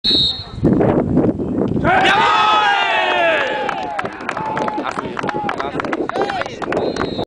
SC Rasdorf-SV Hellas
A-Liga Hünfeld-Hersfeld, 17.04.08
Elfmeter, Michael Stock hält